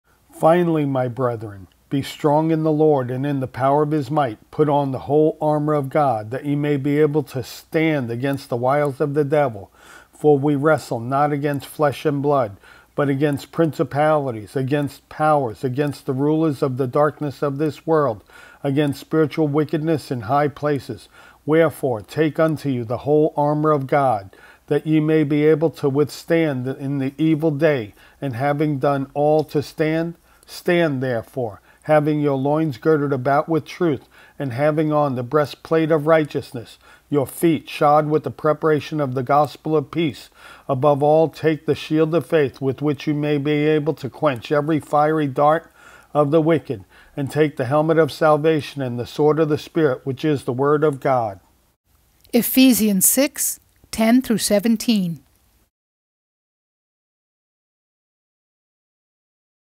Put on the Whole Armor of God - Ephesians 6:10-17
Finally, my brethren, be strong in the Lord, and in the power of his might. Put on the whole armour of God, that ye may be able to stand against the wiles of the devil. For we wrestle not against flesh and blood, but against principalities, against powers, against the rulers of the darkness of this world, against spiritual wickedness in high places. Wherefore take unto you the whole armour of God, that ye may be able to withstand in the evil day, and having done all, to stand. Stand therefore, having your loins girt about with truth, and having on the breastplate of righteousness; And your feet shod with the preparation of the gospel of peace; Above all, taking the shield of faith, wherewith ye shall be able to quench all the fiery darts of the wicked. And take the helmet of salvation, and the sword of the Spirit, which is the word of God: